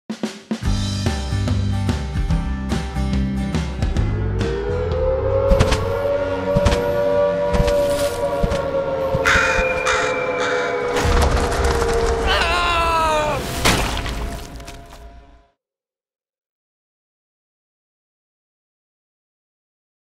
Node Ident 2019 | (Student) Rachel Martin
The inspiration for my Ident came from my partner's father who loves to grow tomatoes, each year the veggie patch is filled with tomato plants, and nothing else, each year he battles with the blackbirds who come and feast on his fruit.